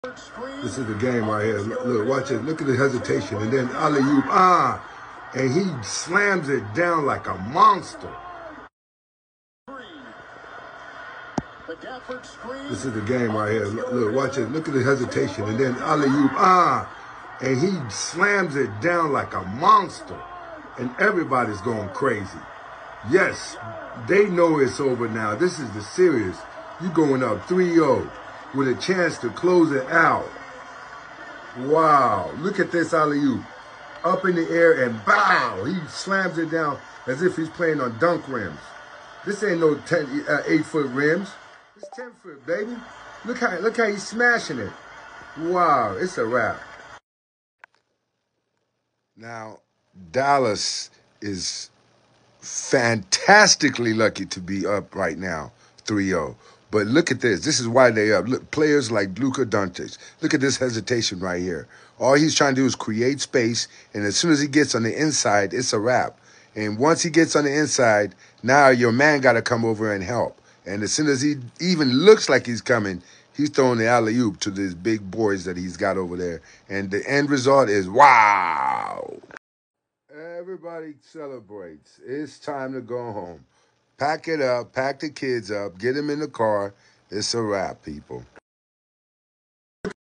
Dallas Mavs Lead Western Conference Finals Series 3-0 Against Minnesota Timberwolves
Luka Doncic and Kyrie Irving scored 33 points apiece and the Dallas Mavericks made all the big plays late again, beating the Minnesota Timberwolves 116-107 on Sunday night for a 3-0 lead in the Western Conference finals.

Thanks